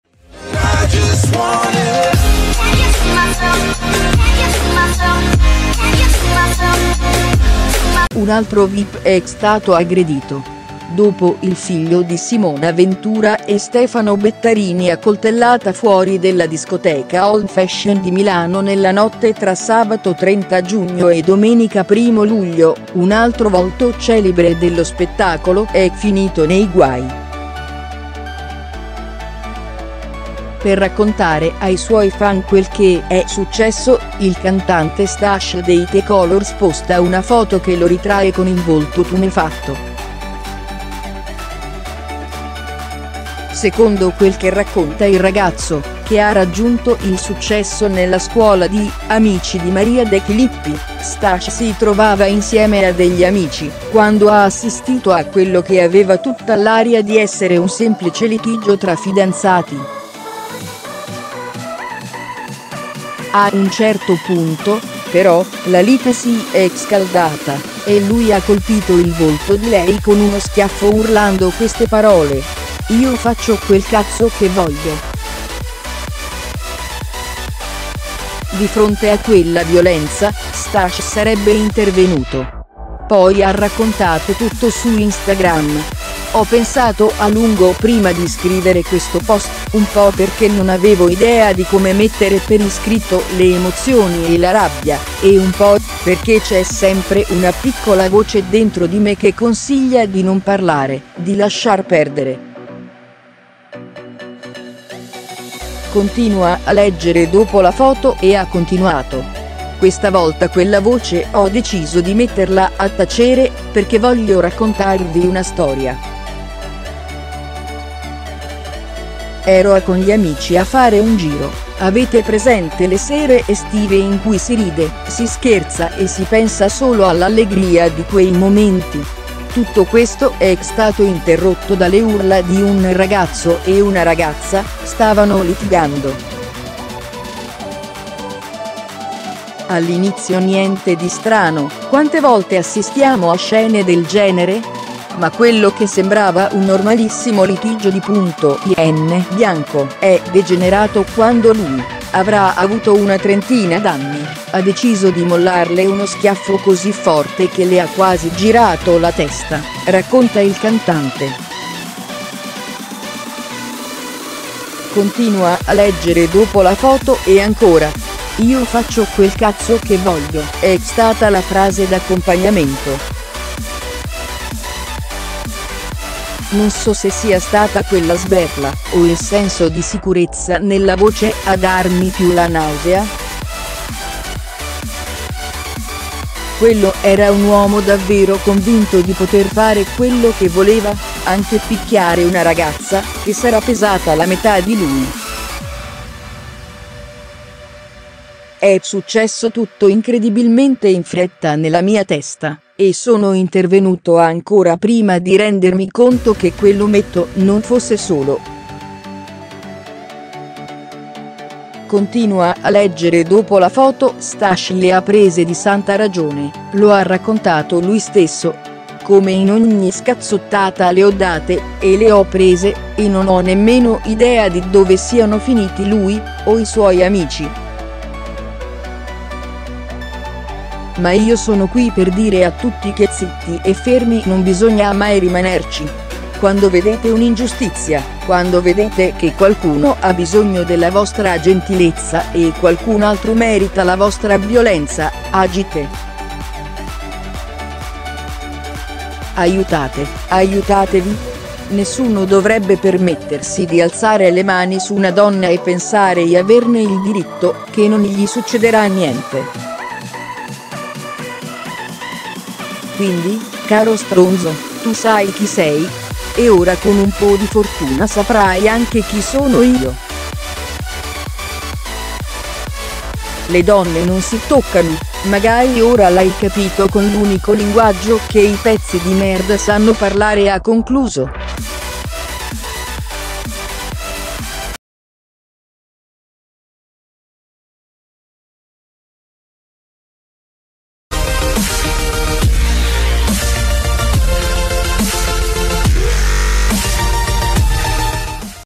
“Preso a pugni in strada”. Un altro vip aggredito. Il racconto è raccapricciante | LE NOTIZIE DEL G
Un altro vip è stato aggredito. Dopo il figlio di Simona Ventura e Stefano Bettarini accoltellata fuori della discoteca Old Fashion di Milano nella notte tra sabato 30 giugno e domenica 1° luglio, un altro volto celebre dello spettacolo è finito nei guai. Per raccontare ai suoi fan quel che è successo, il cantante Stash dei The Kolors posta una foto che lo ritrae con il volto tumefatto. Secondo quel che racconta il ragazzo, che ha raggiunto il successo nella scuola di Amici di Maria De Filippi, Stash si trovava insieme a degli amici, quando ha assistito a quello che aveva tutta l’aria di essere un semplice litigio tra fidanzati. A un certo punto, però, la lite si è scaldata e lui ha colpito il volto di lei con uno schiaffo urlando queste parole: "Io faccio quel cazzo che voglio". Di fronte a quella violenza, Stash sarebbe intervenuto. Poi ha raccontato tutto su Instagram: “Ho pensato a lungo prima di scrivere questo post, un po' perché non avevo idea di come mettere per iscritto le emozioni e la rabbia, e un po' perché c'è sempre una piccola voce dentro di me che consiglia di non parlare, di lasciar perdere”. Continua a leggere dopo la foto   E ha continuato: “Questa volta quella voce ho deciso di metterla a tacere, perché voglio raccontarvi una storia. Ero a con gli amici a fare un giro, avete presente le sere estive in cui si ride, si scherza e si pensa solo all'allegria di quei momenti? Tutto questo è stato interrotto dalle urla di un ragazzo e una ragazza, stavano litigando. All'inizio niente di strano, quante volte assistiamo a scene del genere? Ma quello che sembrava un normalissimo litigio di punto in bianco è degenerato quando lui, avrà avuto una trentina d'anni, ha deciso di mollarle uno schiaffo così forte che le ha quasi girato la testa”, racconta il cantante. Continua a leggere dopo la foto    E ancora: “’Io faccio quel cazzo che voglio’, è stata la frase d'accompagnamento. Non so se sia stata quella sberla o il senso di sicurezza nella voce a darmi più la nausea. Quello era un uomo davvero convinto di poter fare quello che voleva, anche picchiare una ragazza che sarà pesata la metà di lui. È successo tutto incredibilmente in fretta nella mia testa, e sono intervenuto ancora prima di rendermi conto che quell'ometto non fosse solo”. Continua a leggere dopo la foto     Stash le ha prese di santa ragione, lo ha raccontato lui stesso: “Come in ogni scazzottata le ho date e le ho prese, e non ho nemmeno idea di dove siano finiti lui o i suoi amici. Ma io sono qui per dire a tutti che zitti e fermi non bisogna mai rimanerci. Quando vedete un'ingiustizia, quando vedete che qualcuno ha bisogno della vostra gentilezza e qualcun altro merita la vostra violenza, agite. Aiutate, aiutatevi. Nessuno dovrebbe permettersi di alzare le mani su una donna e pensare i averne il diritto, che non gli succederà niente. Quindi, caro stronzo, tu sai chi sei. E ora con un po' di fortuna saprai anche chi sono io. Le donne non si toccano, magari ora l'hai c